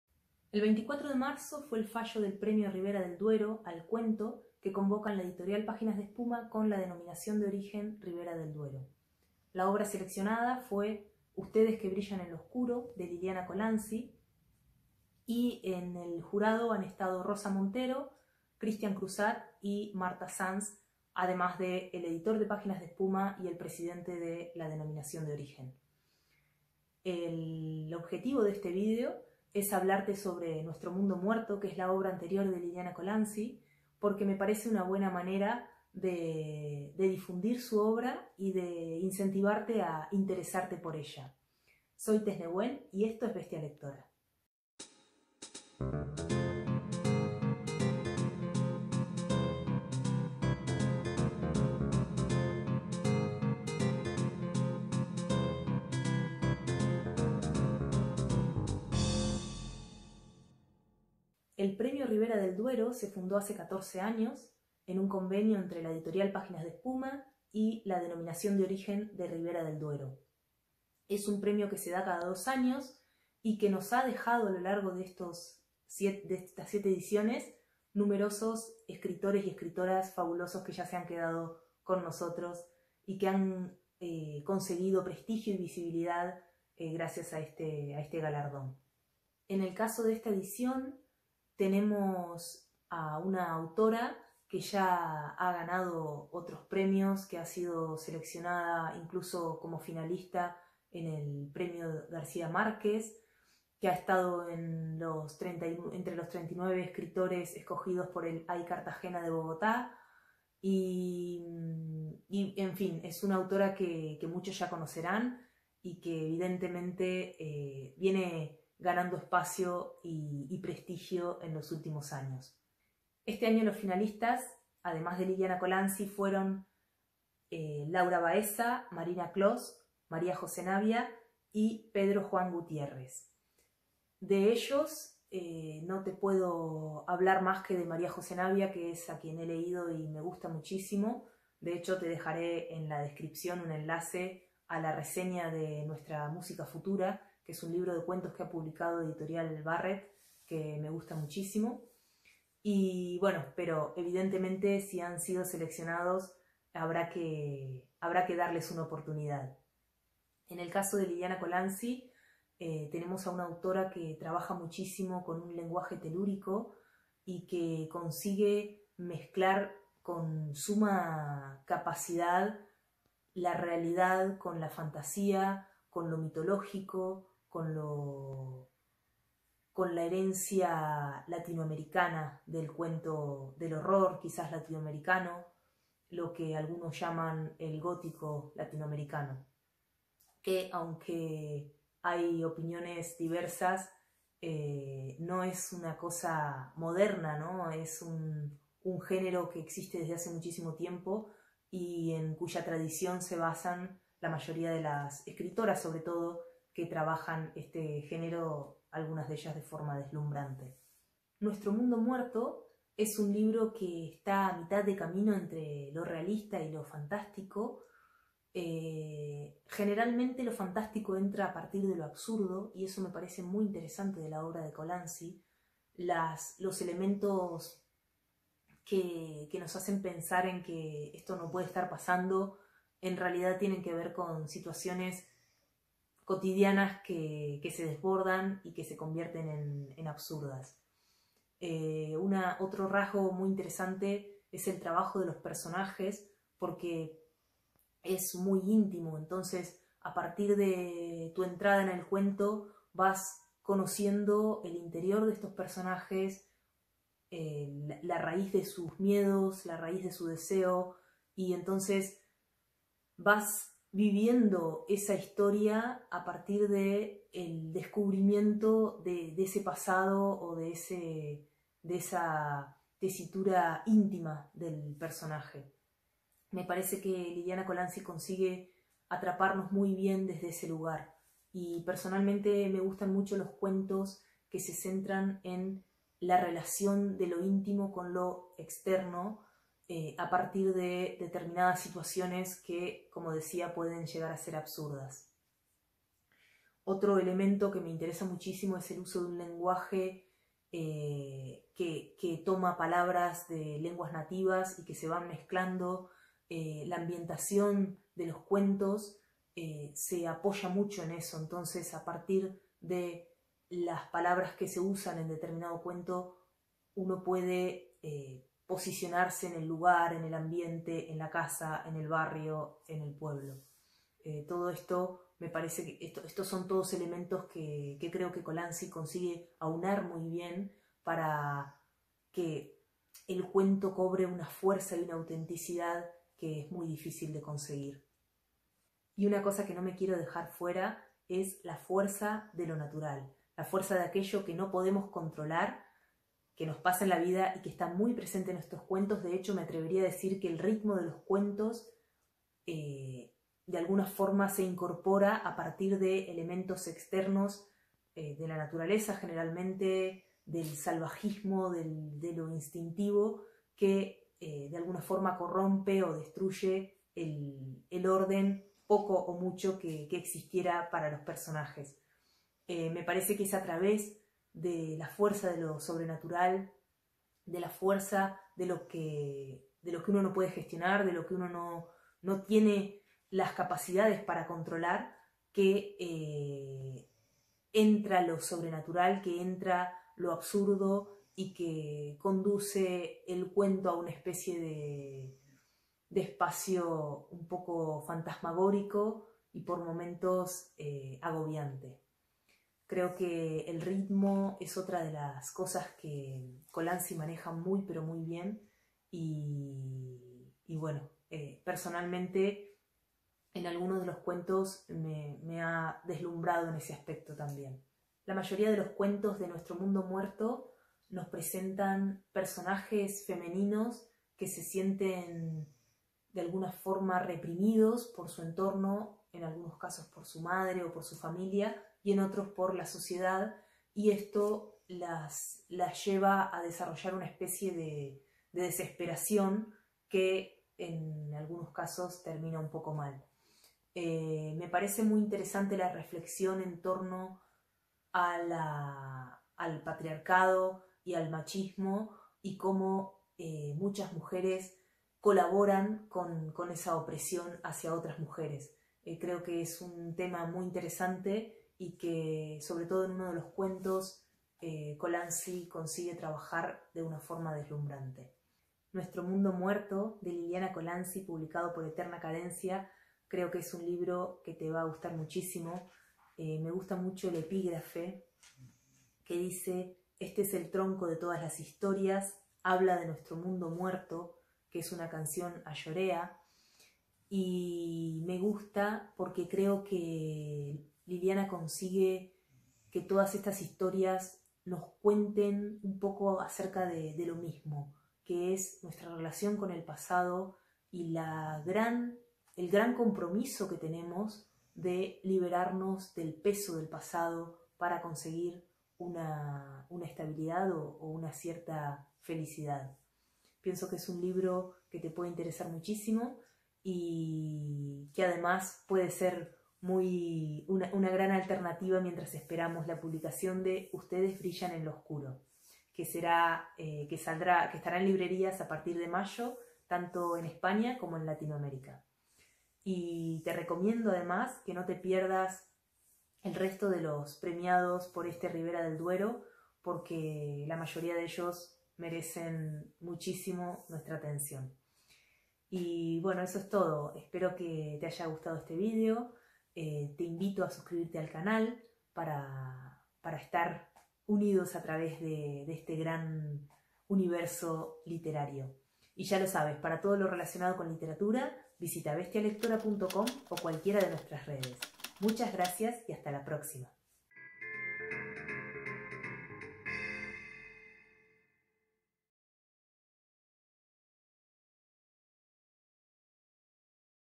Nuestro mundo muerto de Liliana Colanzi
Te hablamos sobre el Premio Ribera del Duero y su más reciente ganadora, la cuentista boliviana Liliana Colanzi. Mientras esperamos la publicación de "Ustedes brillan en lo oscuro" te invitamos a leer "Nuestro mundo muerto", un libro asombroso que puede servirte para conocer a Colanzi.

Reseña de "Una música futura", de María José Navia (Barrett)

Reseña de "La claridad", de Marcelo Luján (Páginas de Espuma)